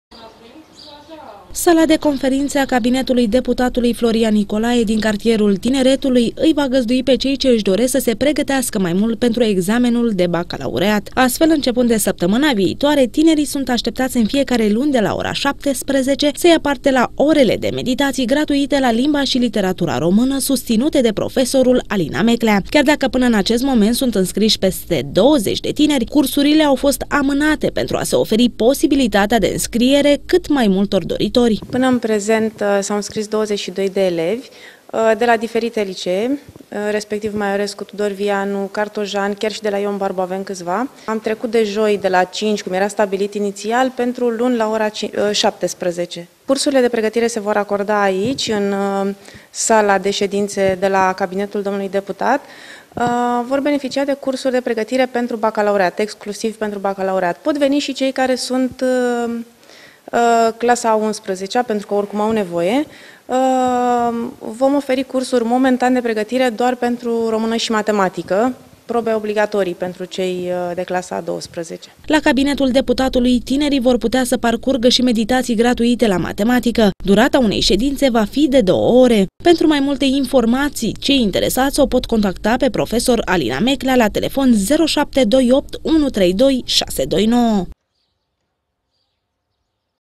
MEDITATII, INCEPAND DE LUNI
Deputatul Nicolae Florian se ține de cuvânt în ceea ce privește oferirea ajutorului său elevilor ce nu au posibilitatea materială să facă meditații pentru bacalaureat. Deși inițial cursurile gratuite erau programate pentru această săptămână, orele de meditații se vor susține la începutul săptămânii viitoare. Peste 20 de elevi din mai multe licee giurgiuvene s-au înscris deja pentru a participa la orele de pregătire gratuită.